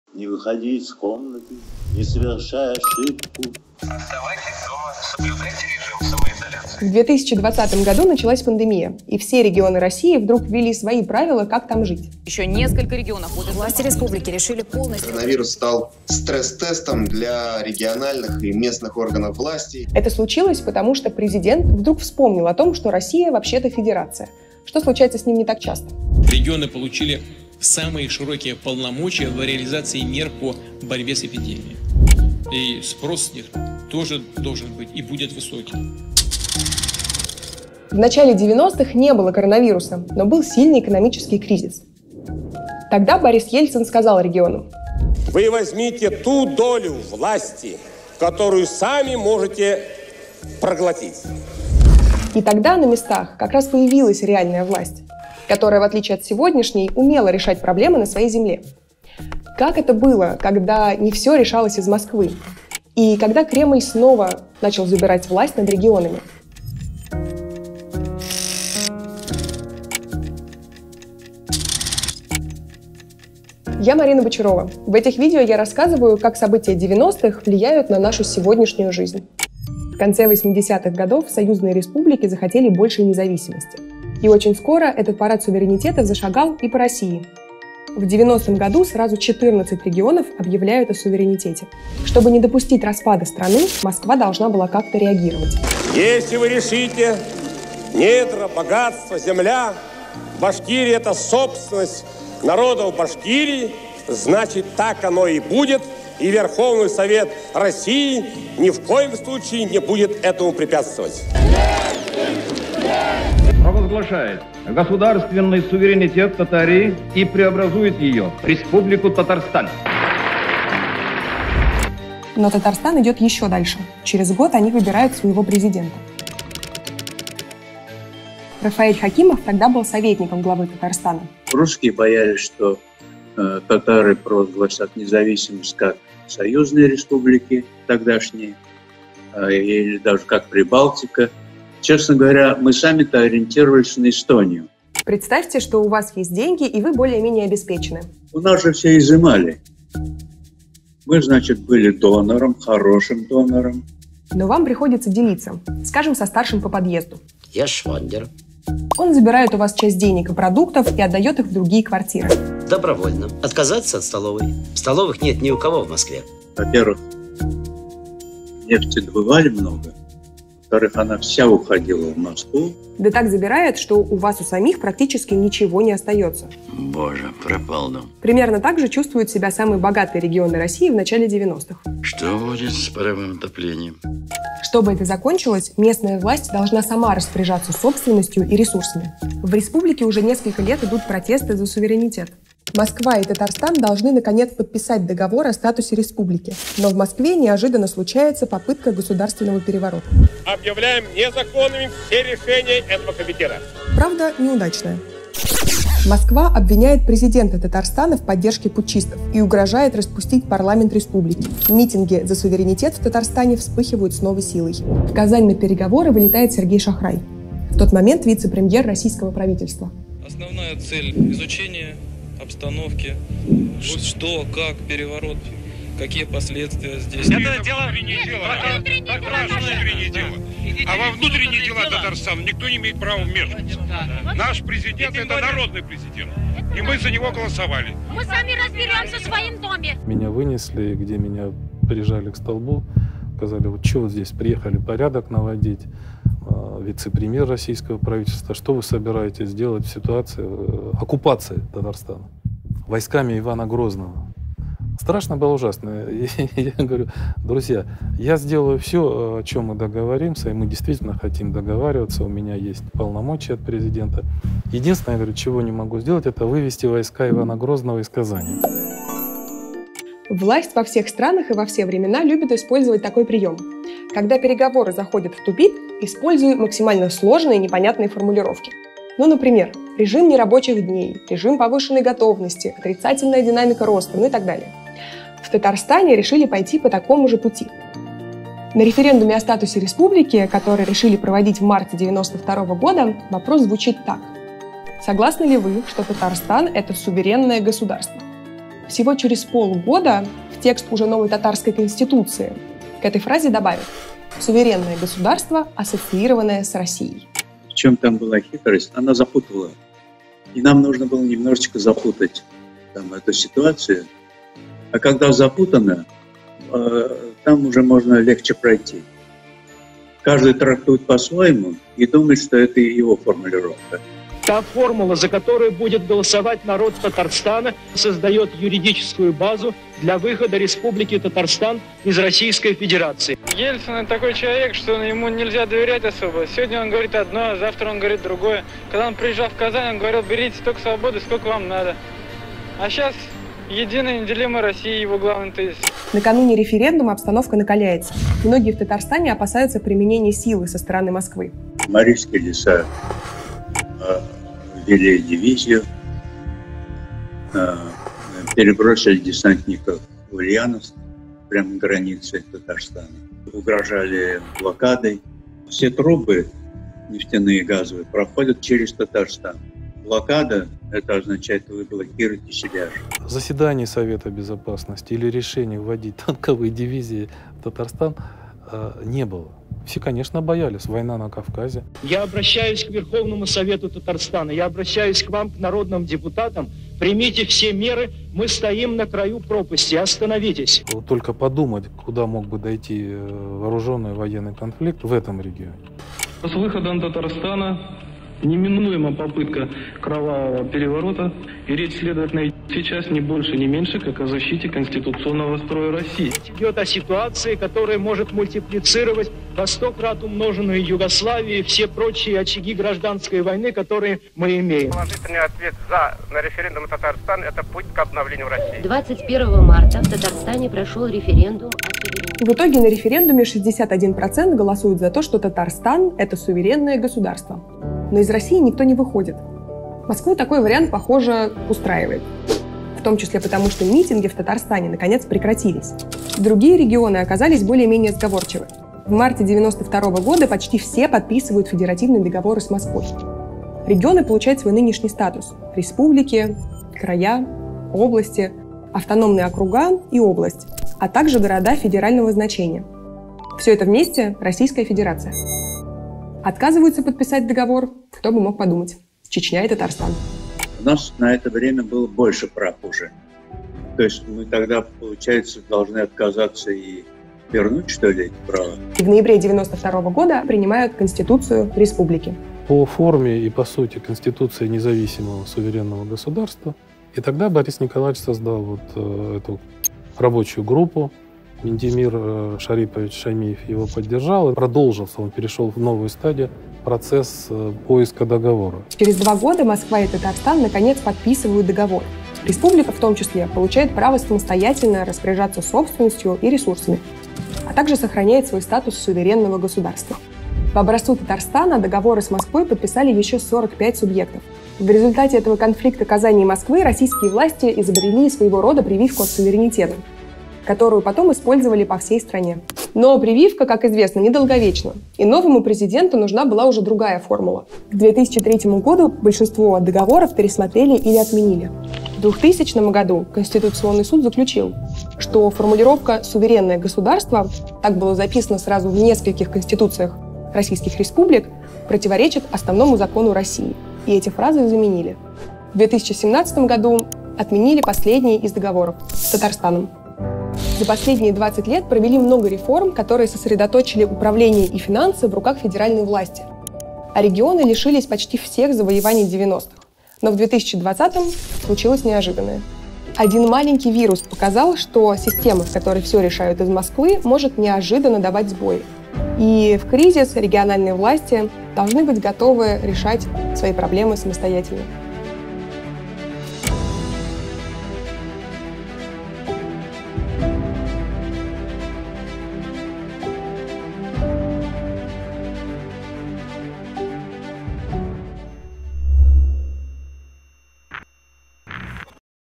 Как Татарстан боролся за суверенитет
В начале девяностых Татарстан попытался стать независимым государством: люди выходили на митинги и требовали экономической и политической самостоятельности. Москва в итоге оставила Татарстану и суверенный статус, и право распоряжаться своими ресурсами и собственностью. Похожие договоры подписали и с другими регионами. Но ненадолго.

Новая власть вскоре договоры отменила, а управление снова начало возвращаться в Москву. И только в 2020 году, из-за пандемии коронавируса, это начало меняться.

«Привет из 90-х» — проект о том, как события первого десятилетия новой России влияют на нашу сегодняшнюю жизнь.